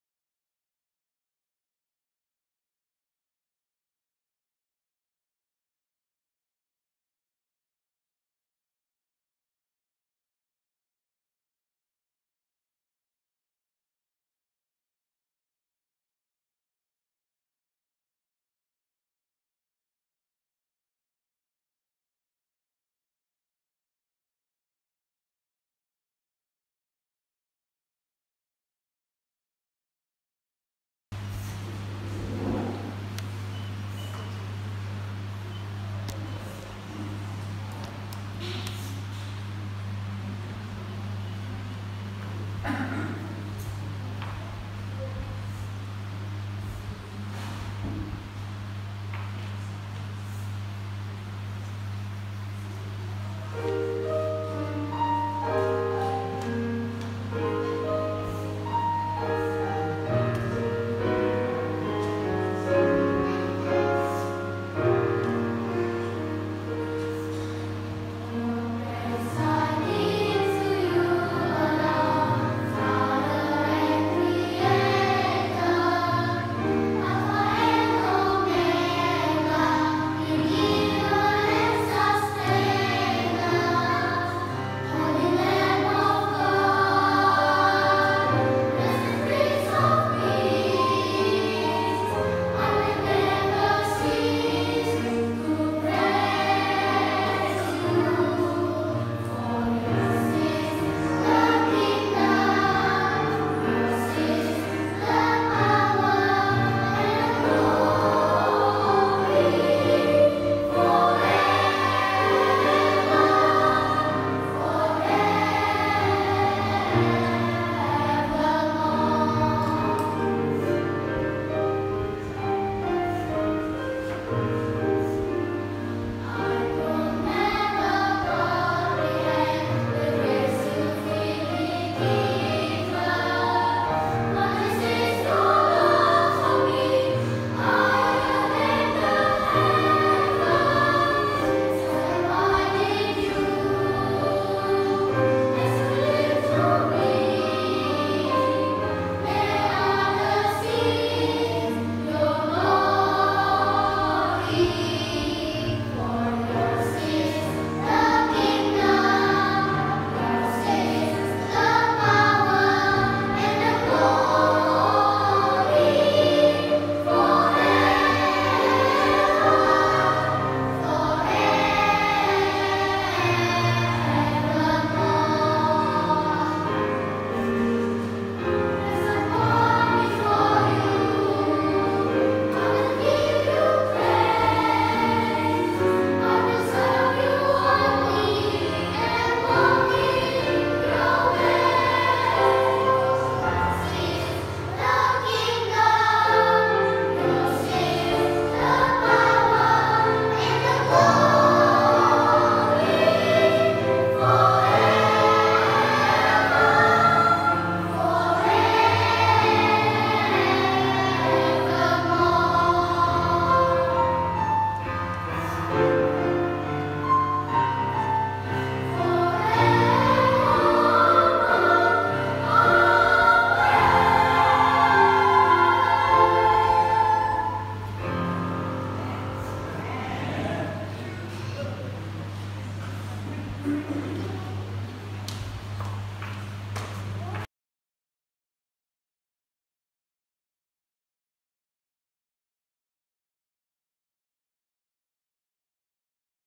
August 31ˢᵗ2019-PCM performing at PAC
Angelic voices from the Apples of Pangasaan Adventist Church

We pray that they will continue to grow in Jesus together with their vocal talents.

Thanks Sir Delmer and Debbie... all parents and all who participated in maintaining the PCM choir, you are all awesome!